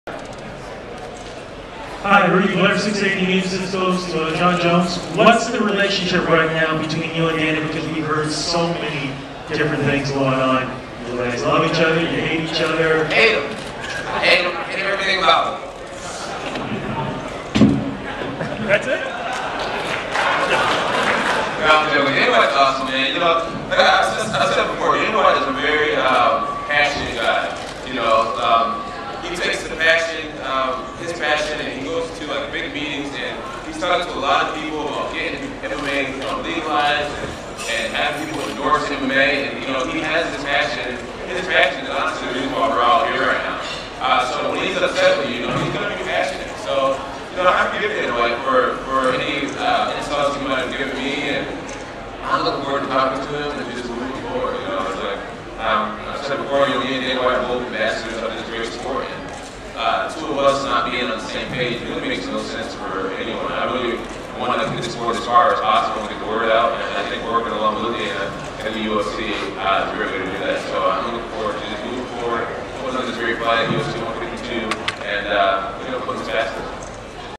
Jon Jones on Relationship with Dana White
Video filmed by me at the 152 card conference.